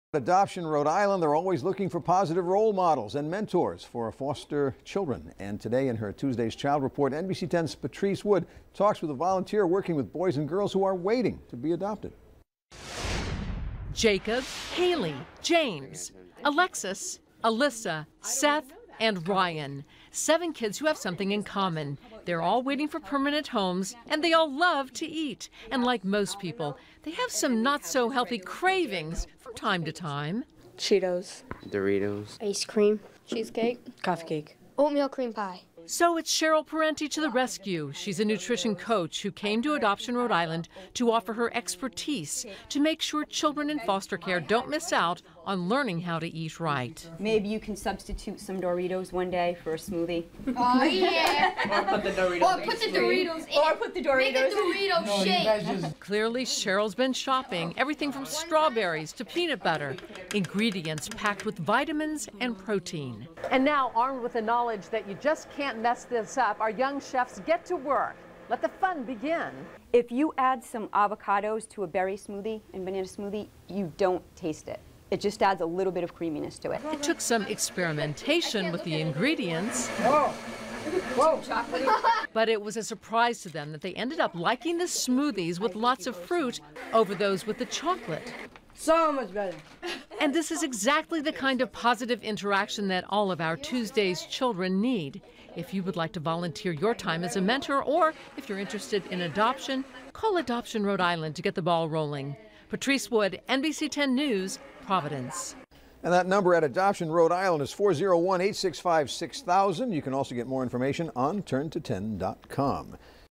Tuesday's Child: Caring volunteers
Adoption Rhode Island is always looking for positive role models and mentors for foster children. In her Tuesday's child report, Patrice Wood finds a volunteer working with boys and girls who are waiting to be adopted.